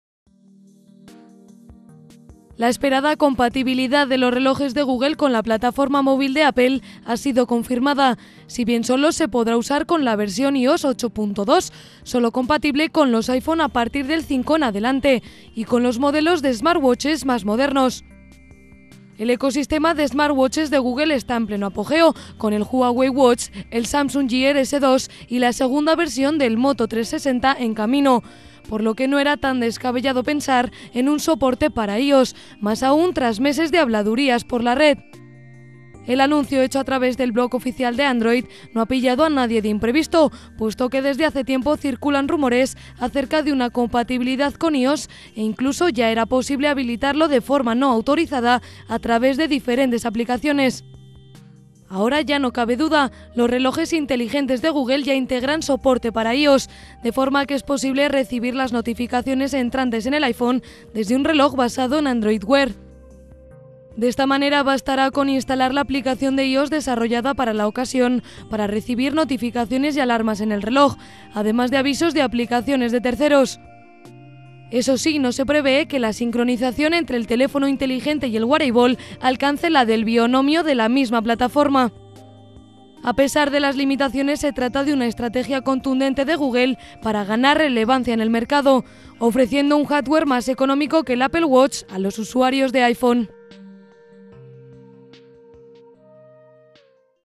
Android Wear ya es compatible con iPhone
La esperada compatibilidad de los relojes de Google con la plataforma móvil de Apple ha sido confirmada, si bien solo se podrá usar con la versión iOS 8.2, solo compatible con los iPhone a partir del 5 en adelante y con los modelos de 'smartwatches' más modernos.